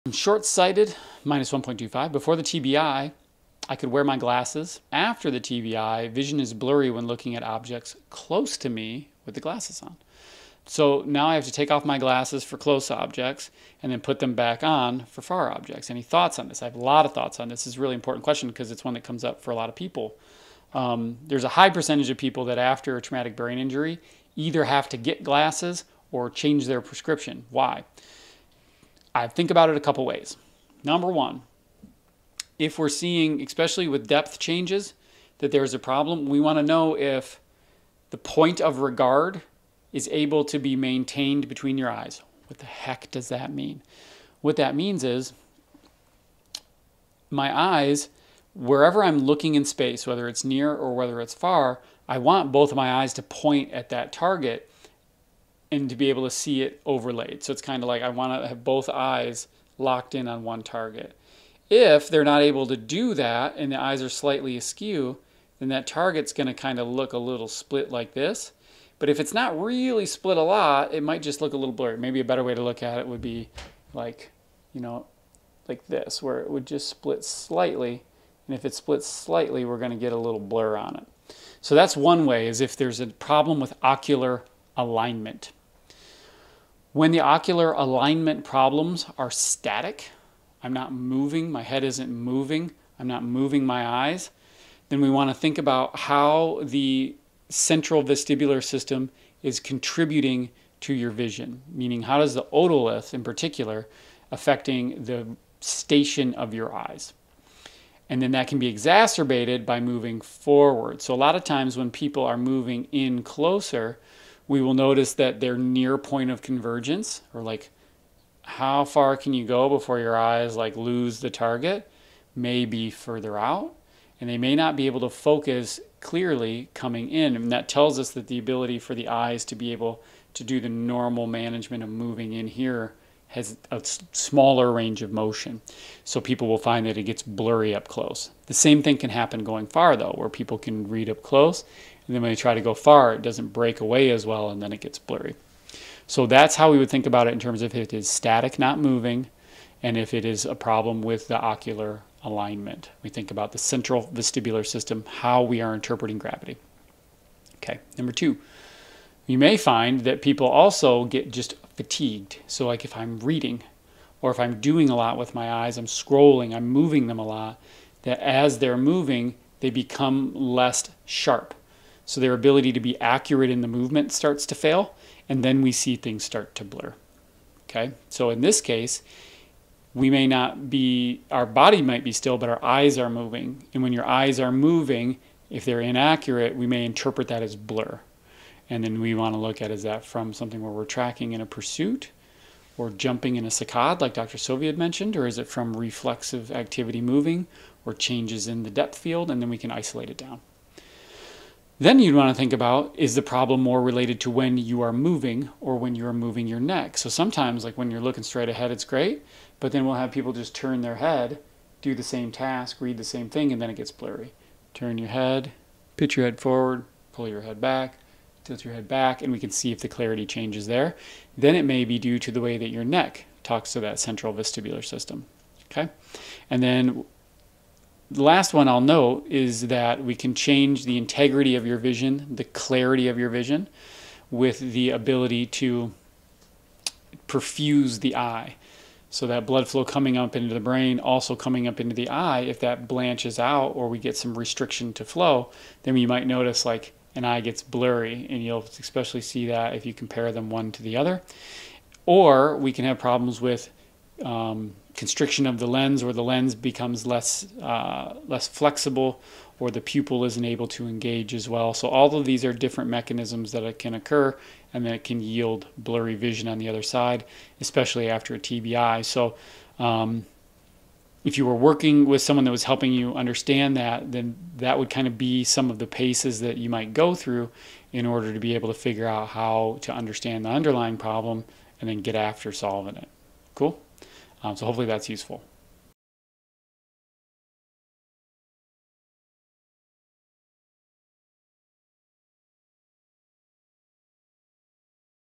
How Dysautonomia Affects Your Eyes
You’re short-sighted  since the TBI, your glasses make close-up work blur. That’s maddening—and common after concussion. In this video I walk through why that happens: tiny misalignments between the eyes can turn a single image into a faint “almost double” that your brain reads as blur; the central vestibular system (especially the otoliths) can tilt the resting posture of the eyes; fatigue in the eye-movement systems (saccades and pursuits) can erode sharpness the longer you read; neck position can hijack clarity; and accommodation/pupil control can change after injury. None of this means your symptoms are “in your head.” It means several systems that used to cooperate are now out of sync.

I’ll show you the same quick checks I use in clinic—near-point of convergence, breaking from near to far and back, simple tracking tasks, and how head pitch or turn alters clarity—and explain what each result suggests. From there, I outline when targeted neuro-optometric/vestibular rehab beats a lens change, and when a small prescription tweak helps you function while we retrain the system. If your near vision is fuzzy with glasses but distance is fine, this will give you a clear, step-by-step way to understand what’s failing and how to rebuild it.

🔹 Our Pledge to You:
Empathy & Compassion